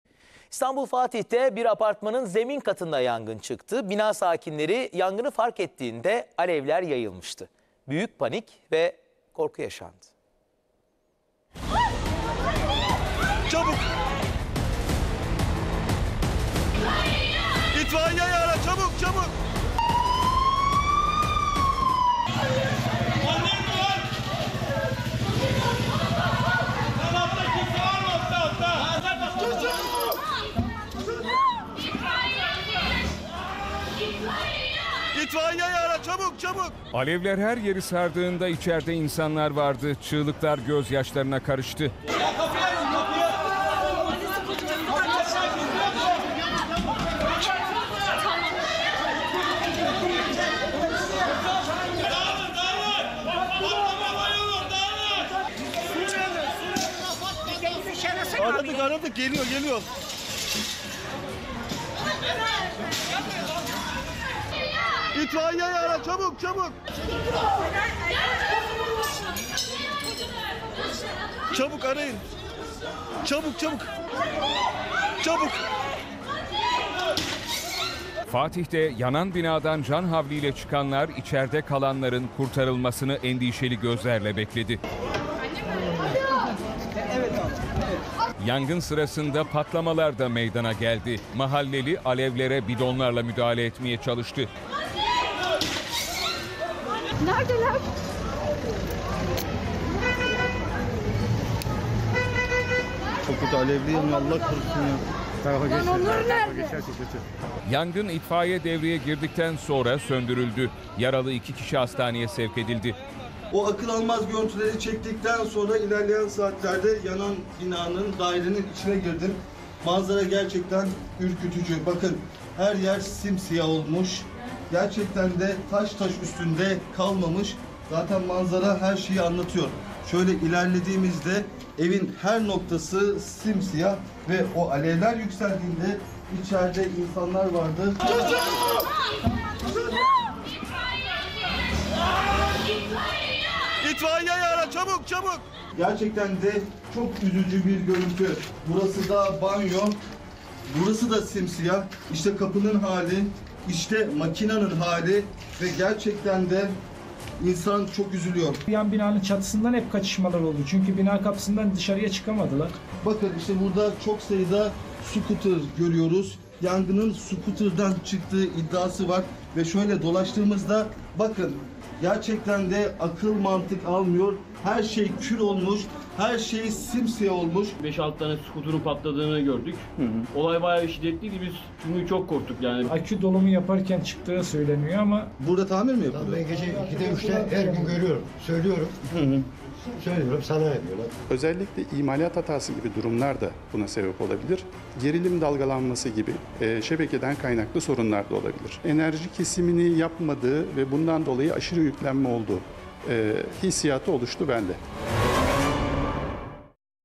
Fatih’te 4 katlı binanın bodrum katında yangın çıktı! | A Haber
İstanbul Fatih’te 4 katlı bir binanın bodrum katında yangın çıktı. 2 kişi dumandan etkilenirken, bodrum katı alev alev yandı.

Yangın, saat 18.45 sıralarında Fatih Sümbül Efendi Mahallesi Kadın Nine Sokak’ta bulunan 4 katlı bir binanın bodrum katında meydana geldi. Henüz bilinmeyen bir nedenle çıkan yangın kısa sürede daireyi sararken, bina sakinlerinin ihbarı üzerine olay yerine itfaiye, polis ve sağlık ekipleri sevk edildi. Dumandan etkilenen 2 vatandaşa sağlık ekipleri tarafından olay yerinde müdahale edildi. Alevler içinde kalan dairedeki yangın, itfaiye ekiplerince kontrol altına alınarak söndürüldü.